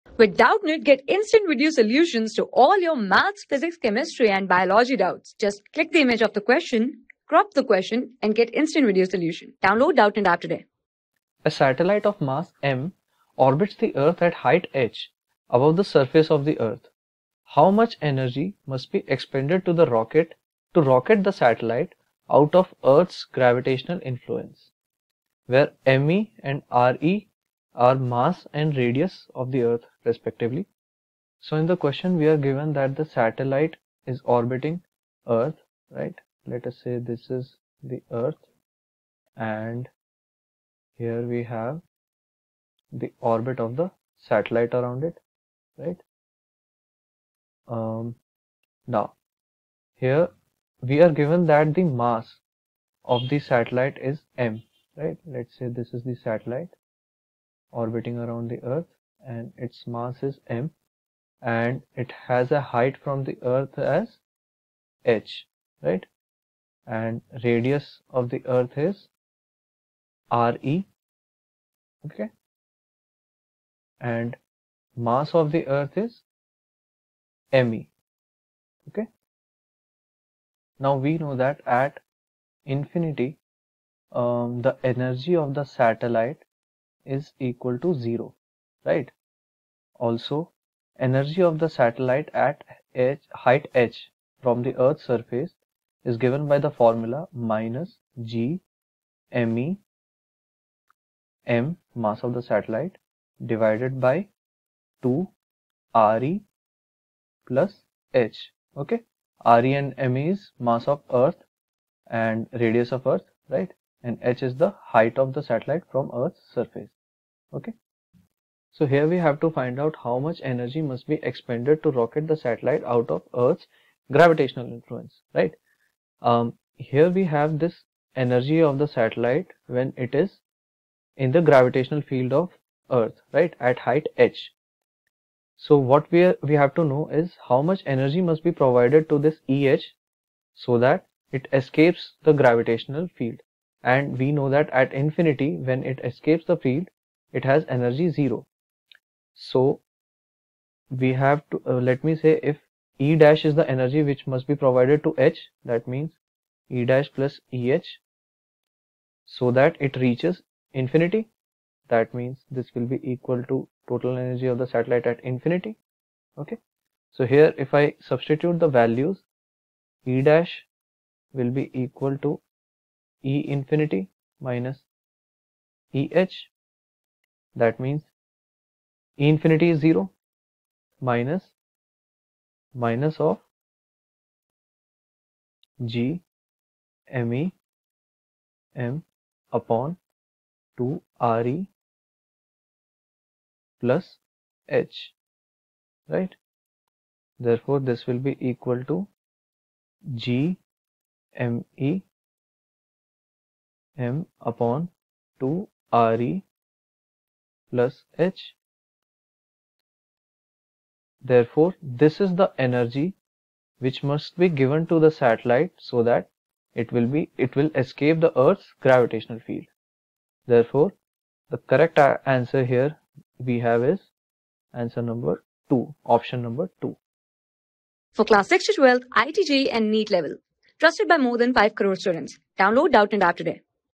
A satellite of a mass m orbits the earth at a height h above the surface of the earth. How much ...
A satellite of a mass m orbits the earth at a height h above the surface of the earth. How much energy must be expended to rocket the satellite out of earth\'s gravitational influence? (where M_(E) and R_(E) be mass and radius of the earth respectively)
Class: 11
Subject: PHYSICS
Chapter: GRAVITATION
Board:NEET

👉 Have Any Query? Ask Us.